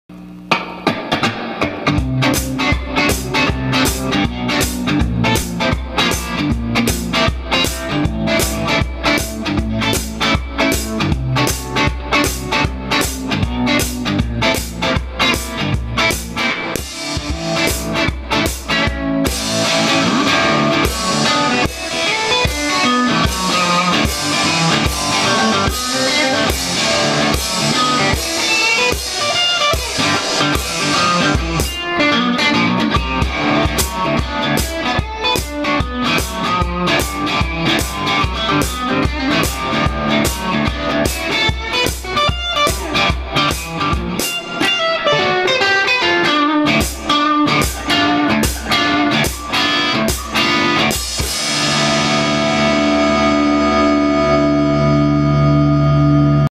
Thi-Hat swing on Stagg 12" Black Metal hi-hat cymbals with cajon and telecaster
a demo of thi-hats using 12" Stagg Black Metal hi-hat cymbals with guitar and cajon after a mean workday...